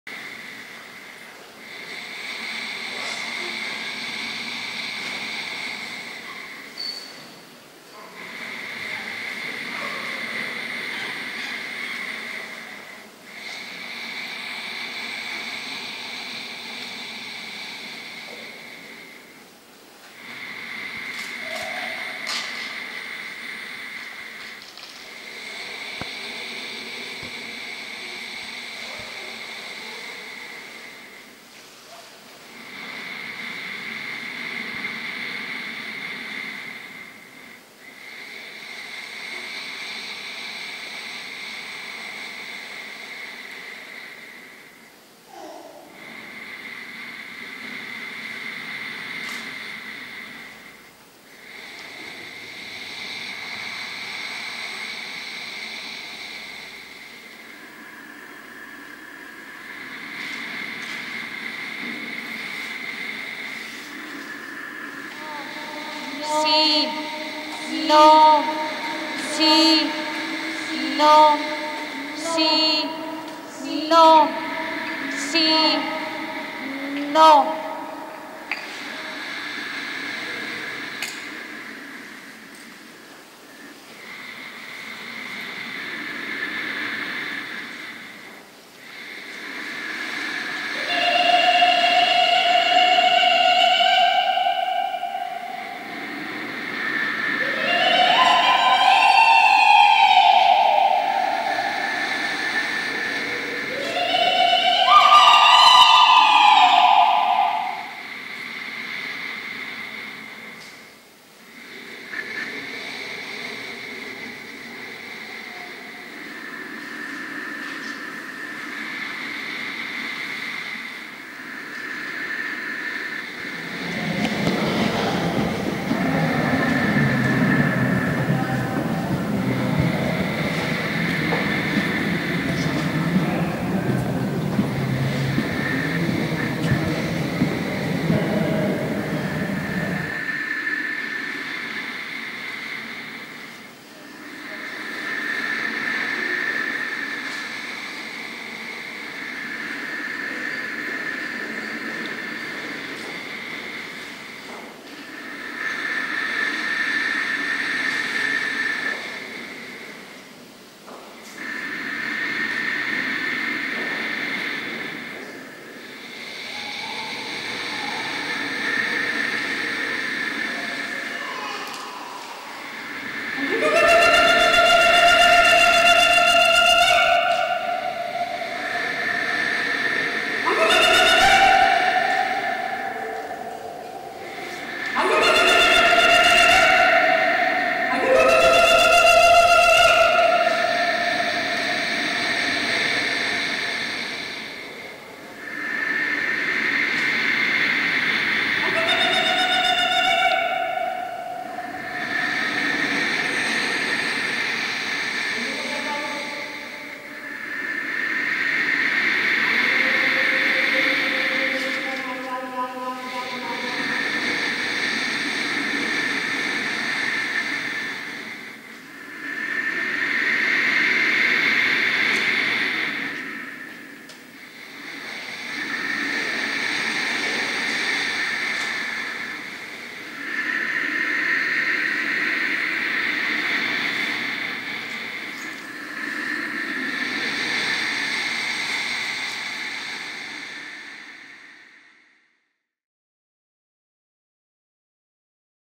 Itziar Okariz
Imatges i textos de l'exposició Respiració i altres sons d'Itziar Okariz al Centre d'Art La panera de Lleida